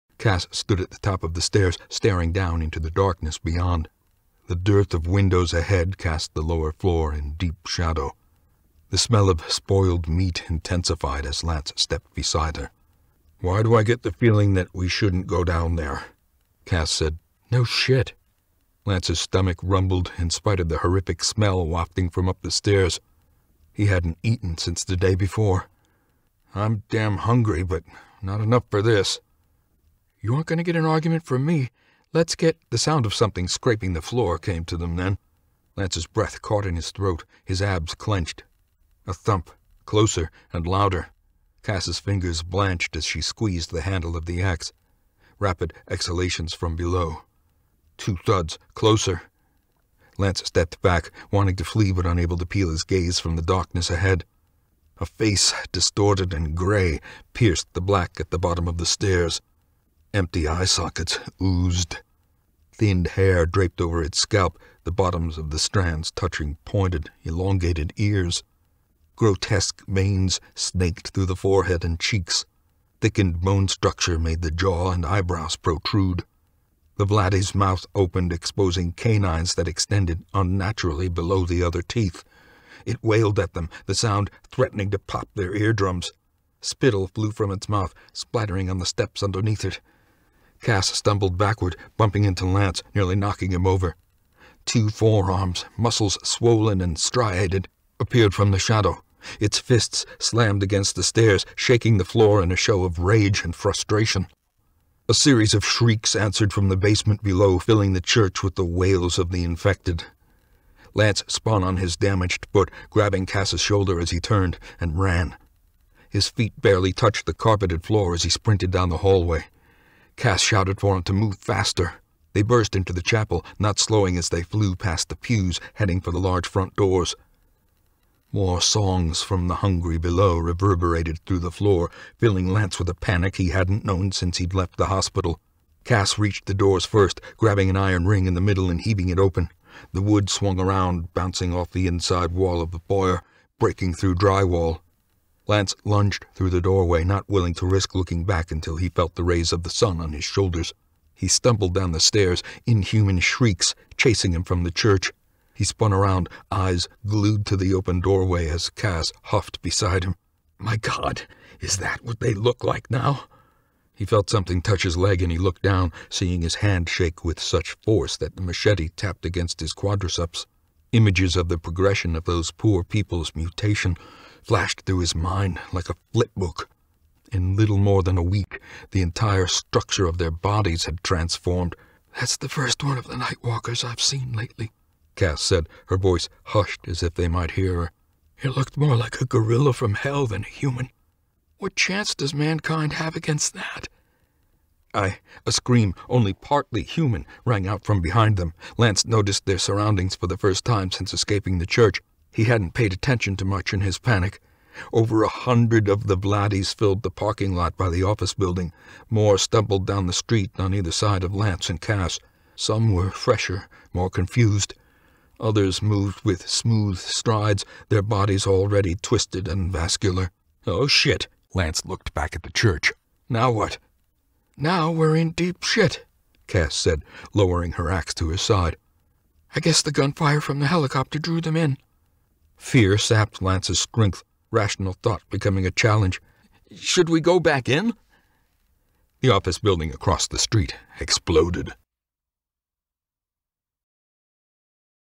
Devoured (The Hunger #1) Audiobook by Jason Brant read by Wayne June
Life isn't kind to Lance York. A full-time job has eluded him for years, his wife loathes the sight of him, his bank accounts are empty, and his wealthy father-in-law revels in his failures.

After he lunges in front of a car to save a sick and disoriented woman, Lance awakens in a quarantined hospital. A devastating plague is spreading worldwide, driving those infected with it insane. Their bodies begin to mutate into horrors that have haunted mankind's nightmares for centuries.

The world descends into chaos as death holds sway in the streets.

With the help of an unlikely ally, Lance must navigate through the collapsing city of Pittsburgh, striving to escape the madness of the Apocalypse that unfolds around them.

Consumed (The Hunger #2) is now available!

My mailing address:
Jason Brant
Abingdon, MD 21009